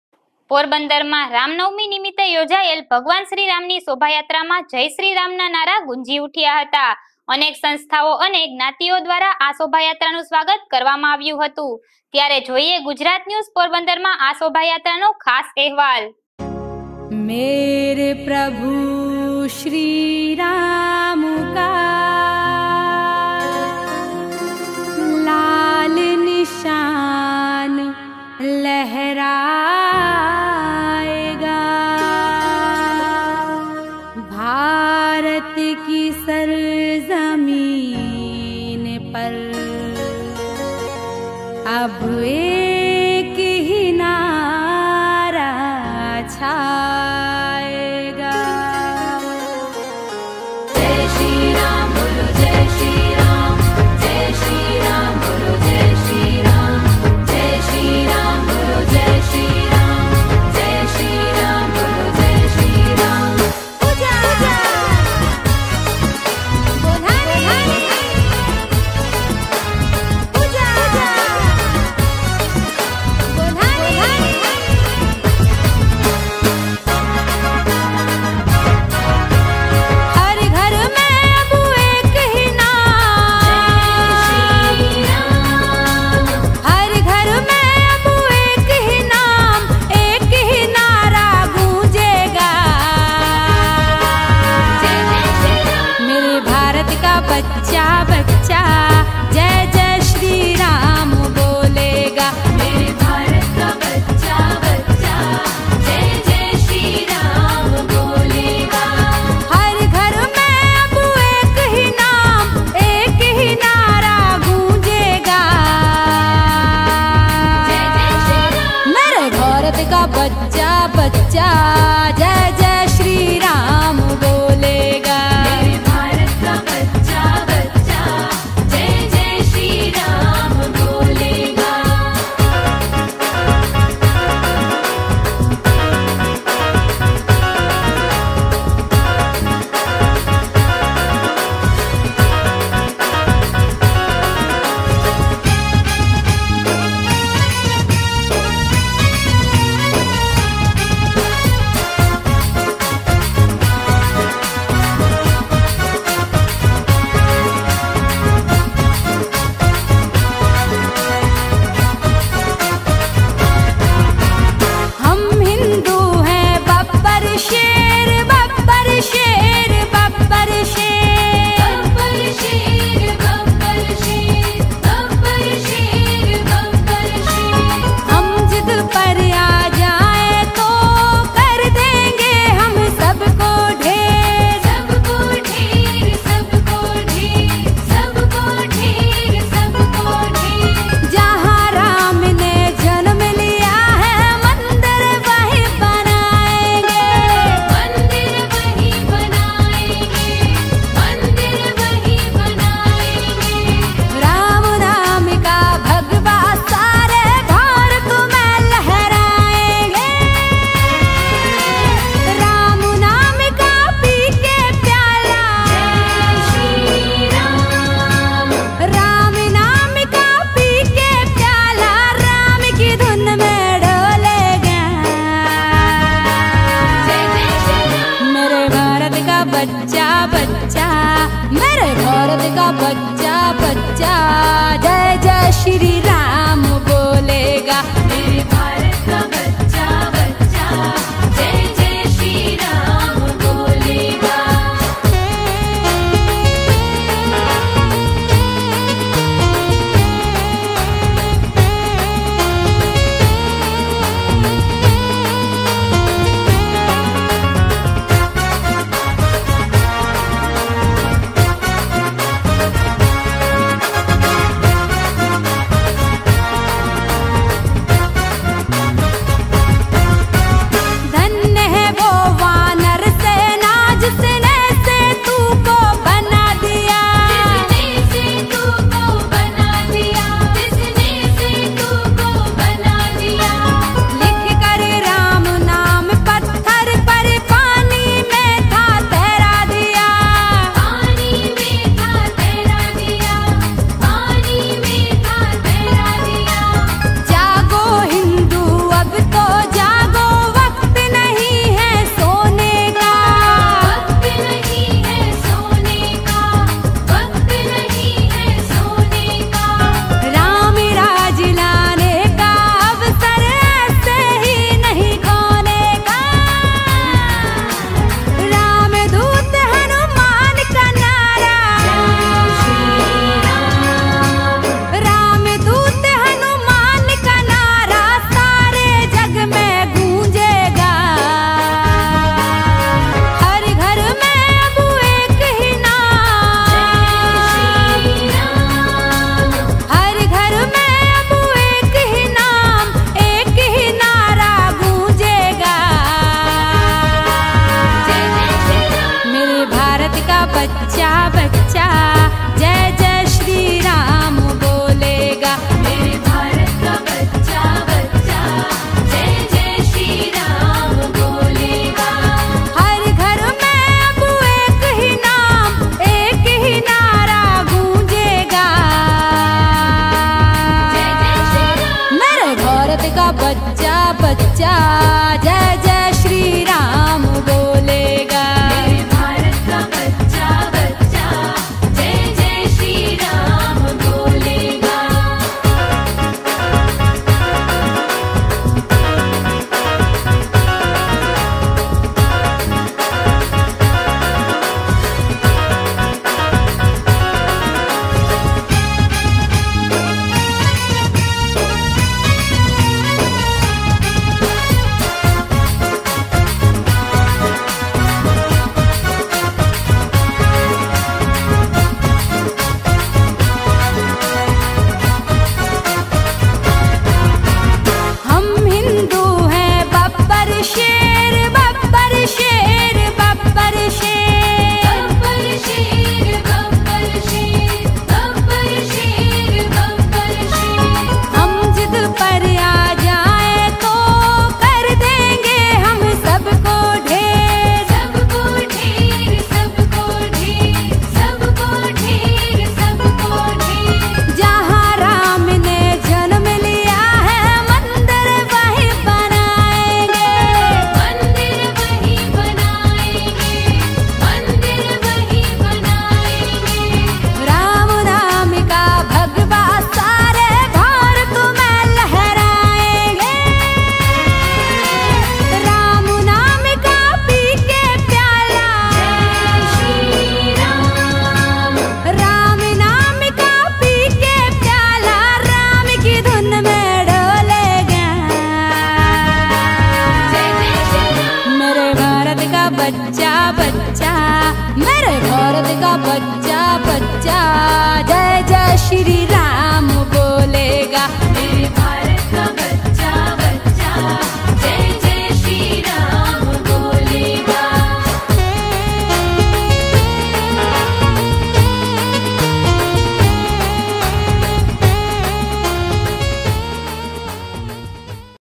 PORBANDAR ગુજરાત ન્યૂઝ-પોરબંદરમાં શ્રી રામની શોભાયાત્રાનો ખાસ અહેવાલ 10-04-2022
Download Gujarati Dayro App